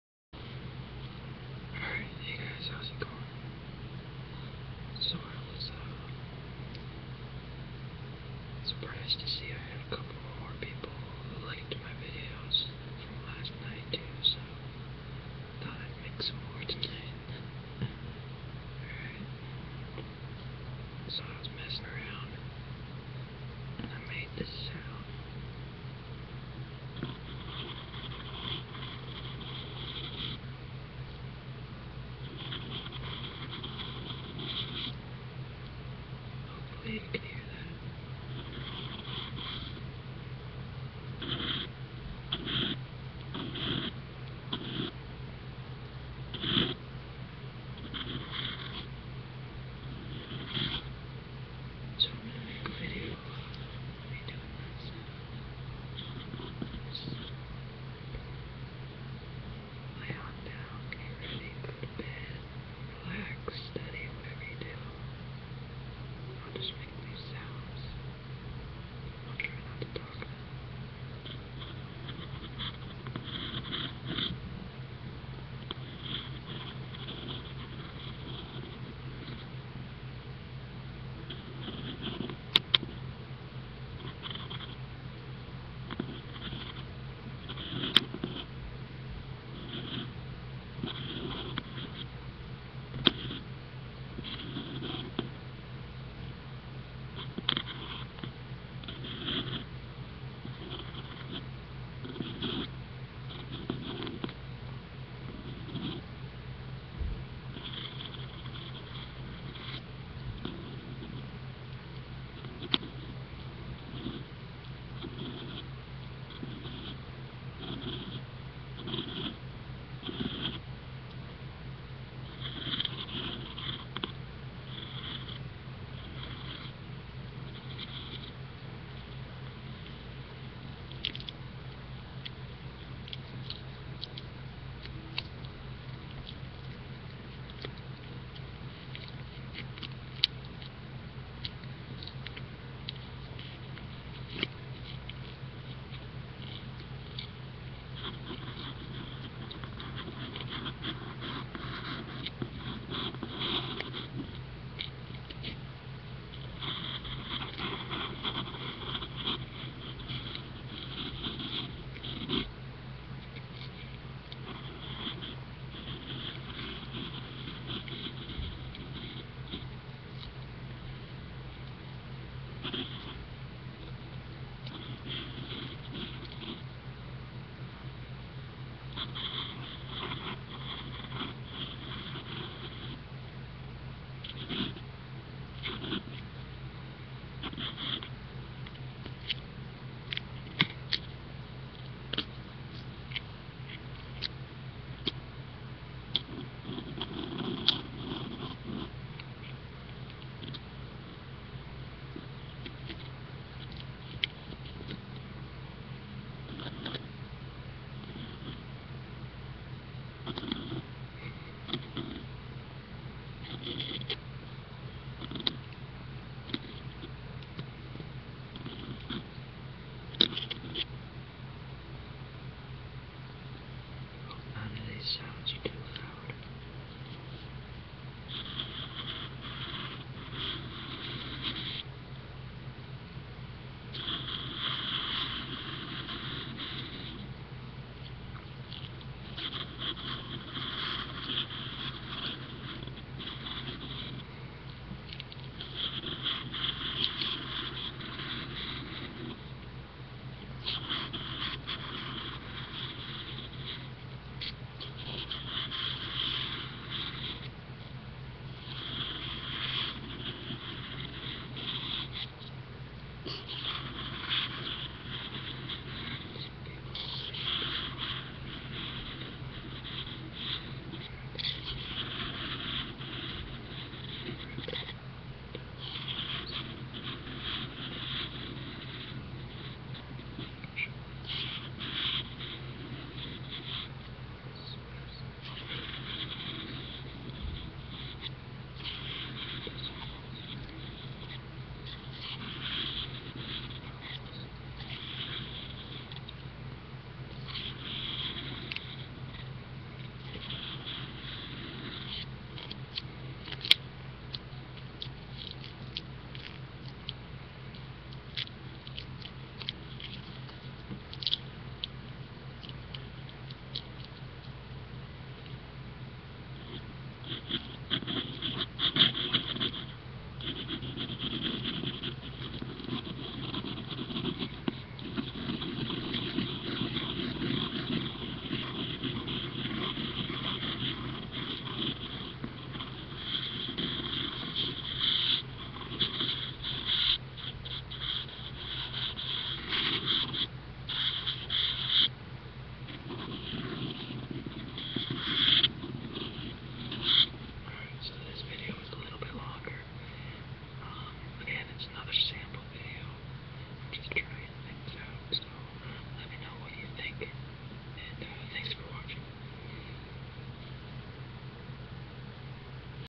ASMR Scratching, Crinkle, Ambient Background. Sounds Mostly. Enjoy
I am new at this. Let me know what you think =) ASMRJordan